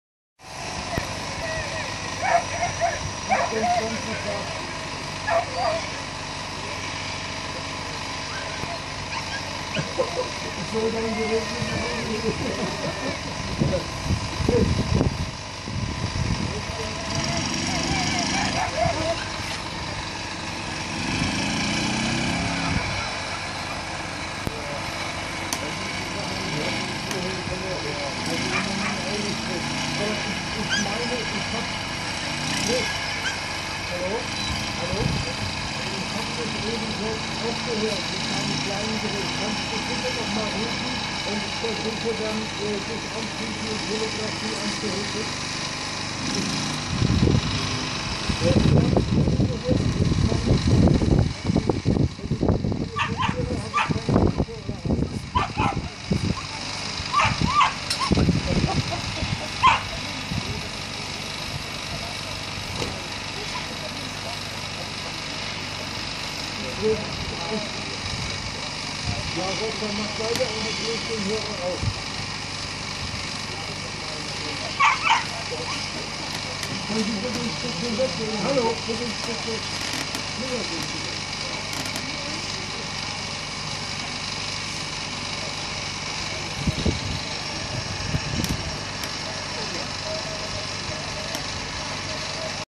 Internationales Coursing Großenaspe, nationale Klasse Whippet Rüden
Unter weiß Akinlana Olabisi